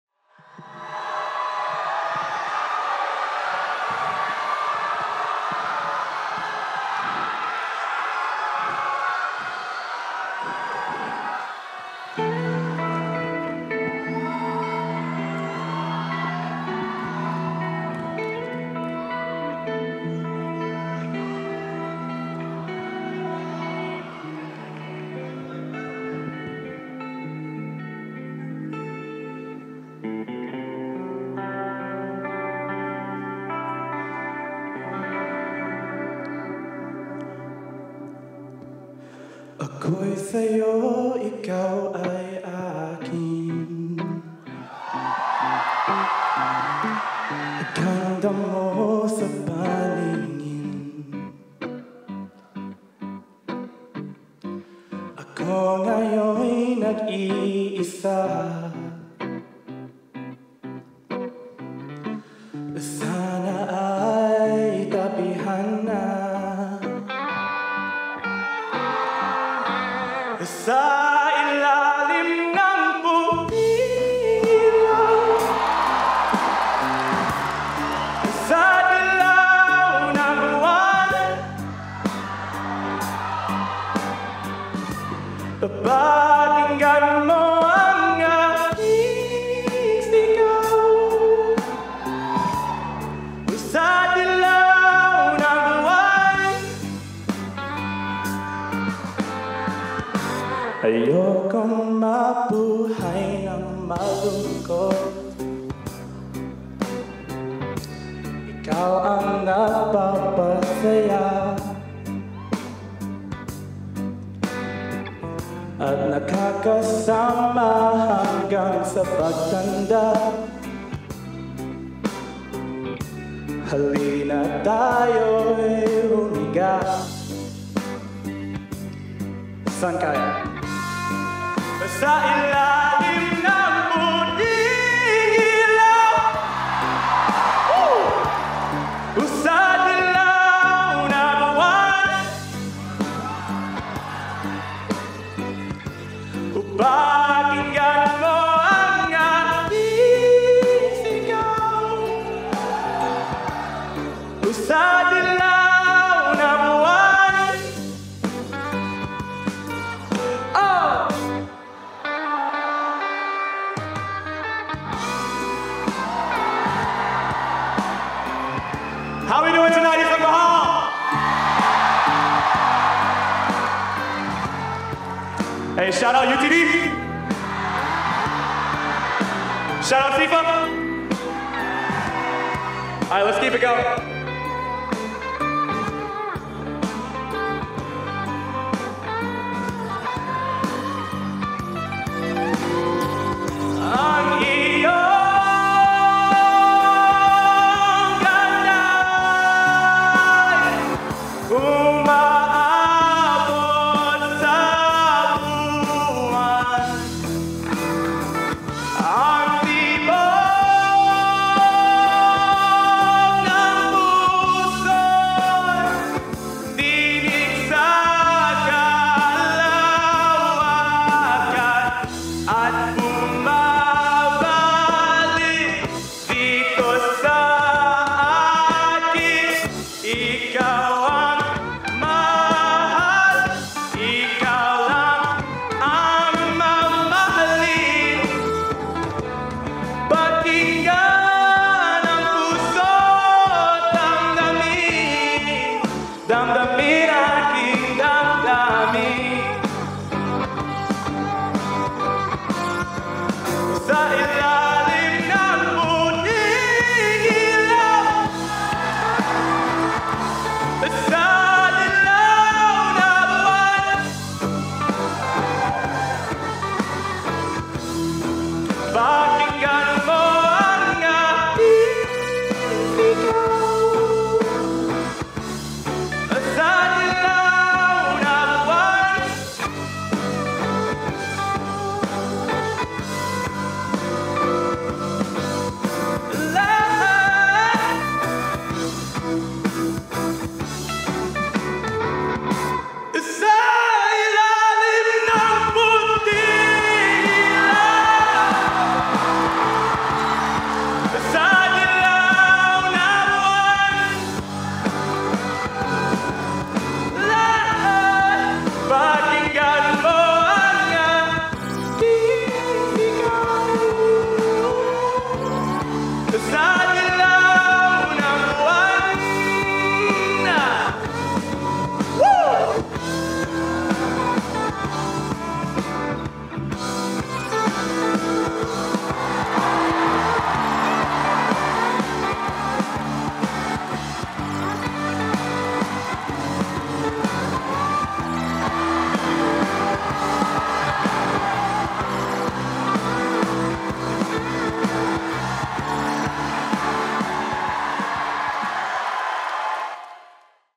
Alvin Bagaoisan | Isang Mahal 2024 [FRONT ROW]
Alvin Bagaoisan | Vocal Performance (3RD PLACE)

Media Coverage by: Paul Young

Isang Mahal "One Love" Talent Show 2024 Hosted by TAMU PhilSA